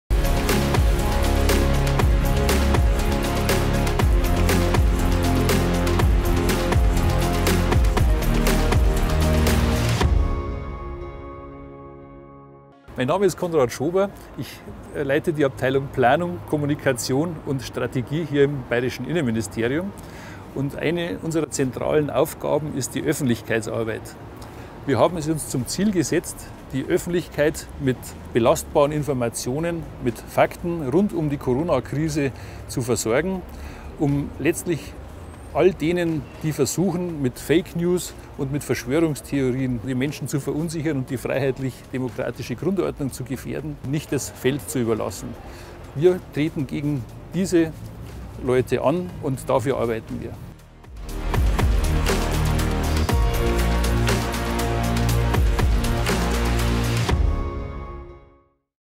92 Tage Corona-Katastrophenfall – Konrad Schober
92 Tage Katastrophenfall: Vom 16. März bis 16. Juni 2020 arbeiteten die Behörden in Bayern Hand in Hand mit den Einsatz- und Rettungsorganisationen, um den Kampf gegen das Corona-Virus aufzunehmen und Menschenleben zu retten. Unsere Videos werfen Schlaglichter auf die Gesichter der Führungsgruppe Katastrophenschutz Bayern während der Corona-Pandemie 2020.